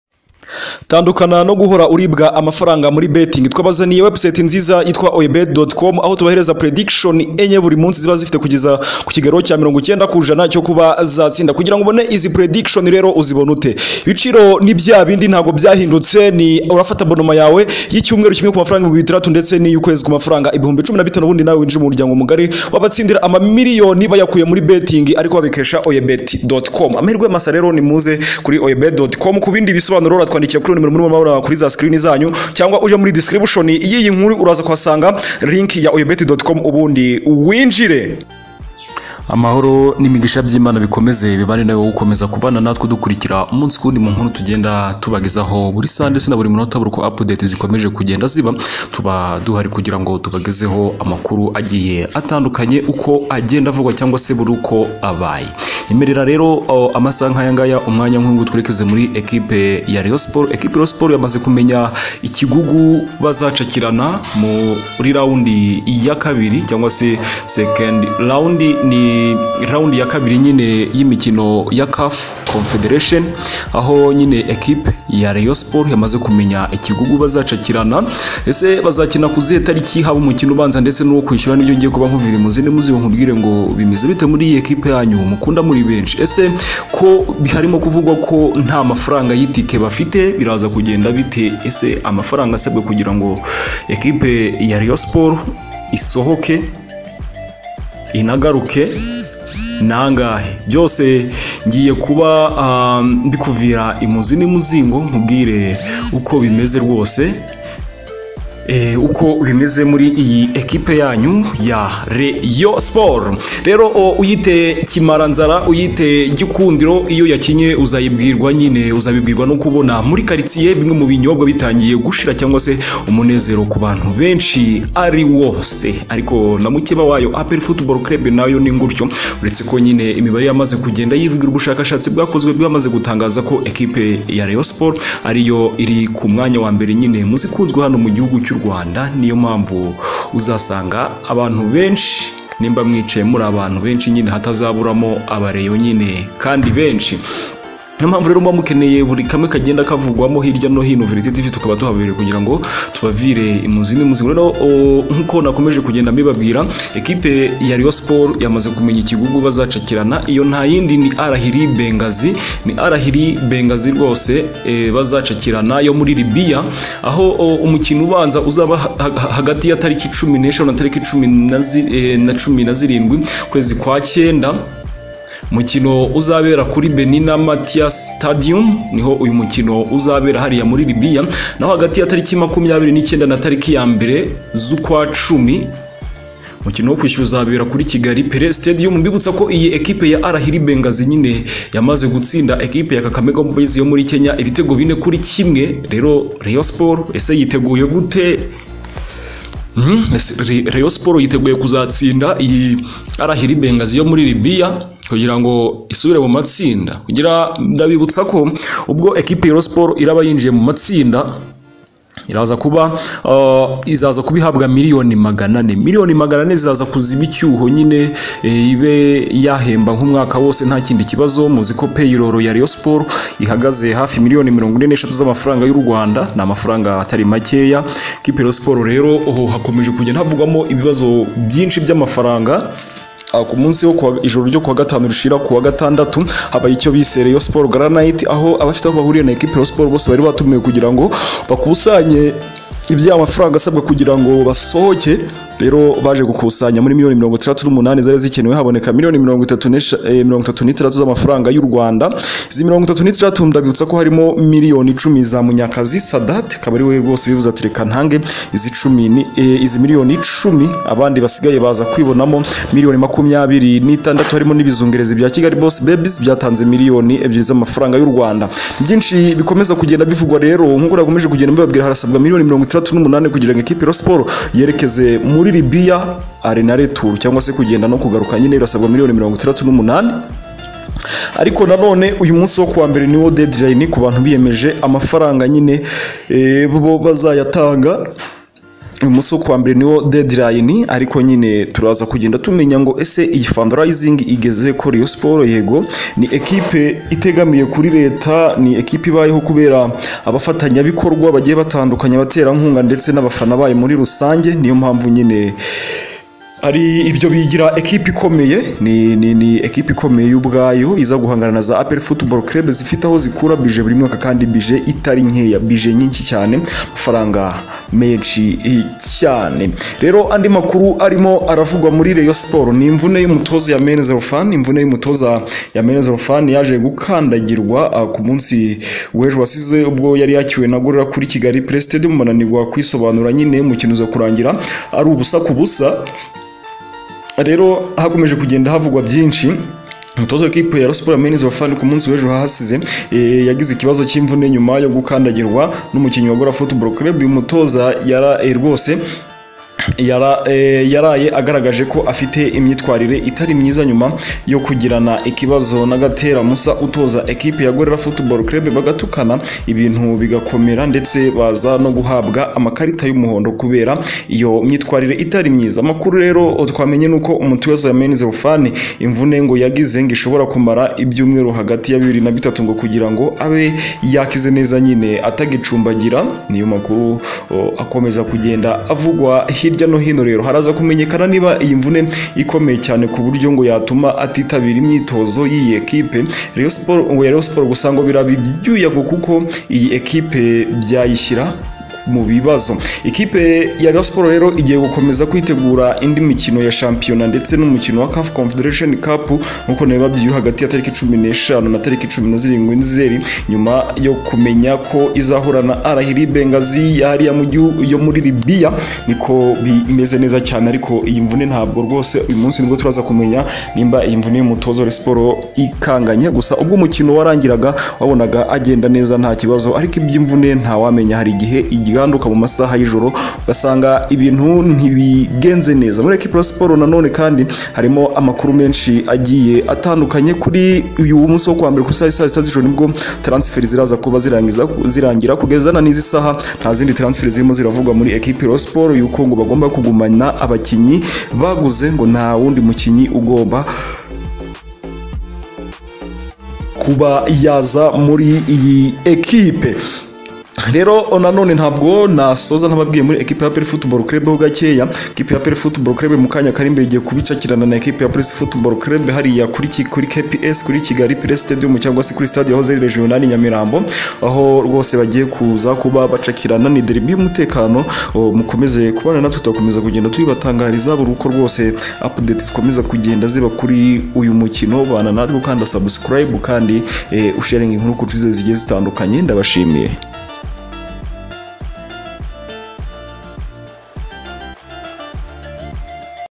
RAYON SPORTS YAMENYE IKIPE YIKIGUGU BAZESURANA MURI CAFC//TAYARI AMATSINDA YAHUMUYE
@VERITE TV
YOUR HOME OF SPORTS NEWS